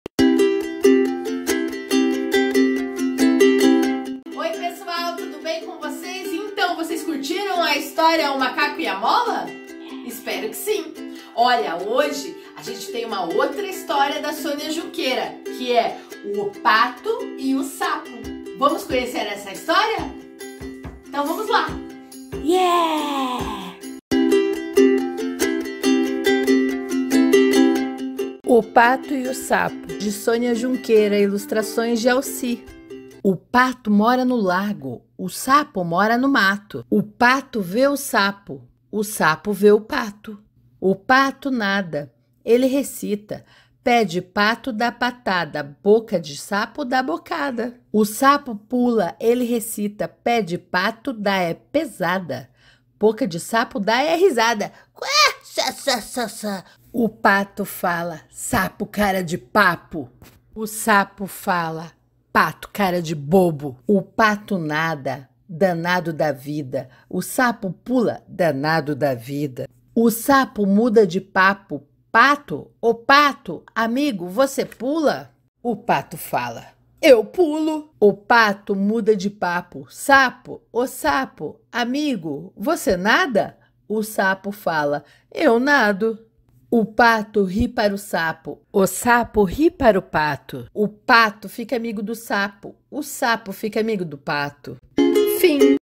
14. Caixa de Autores - Sonia Junqueira - O pato e o sapo (2)
O vídeo de hoje traz a narração da segunda história da autora: Sônia Junqueira - O pato e o sapo.
Vocês vão adorar!